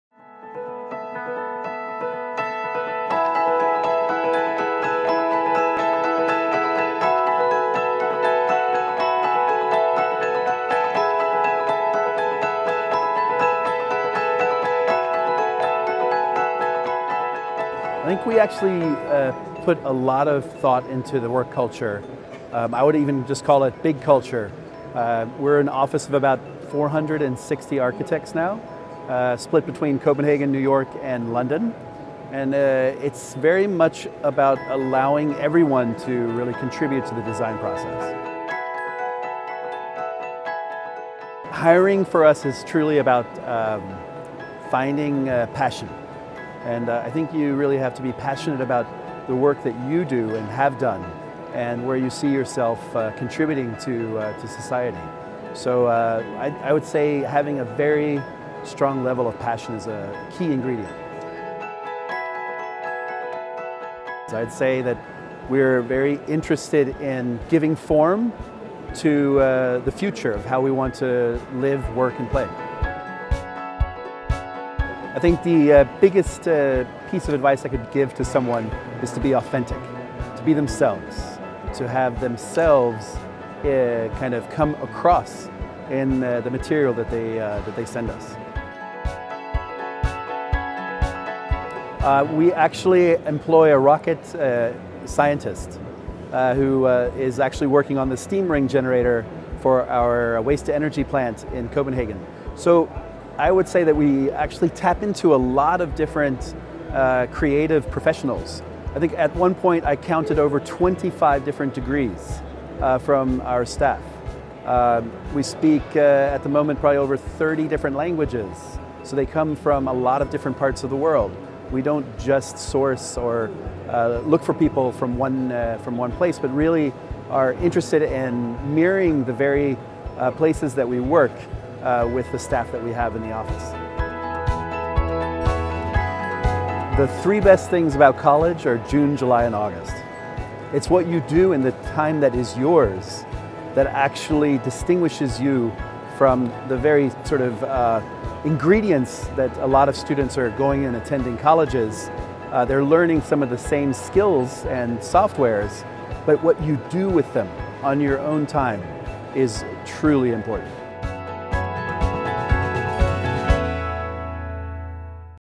"Finding passion" | Kai-Uwe Bergman - BIG
Filmed at the AIA Conference in Orlando, Kai-Uwe Bergmann, Partner at BIG, talks to Bespoke Careers about their office culture and design ethos, what makes a good first impression,  what makes someone a BIG person, and his advice to someone starting out in their architecture career.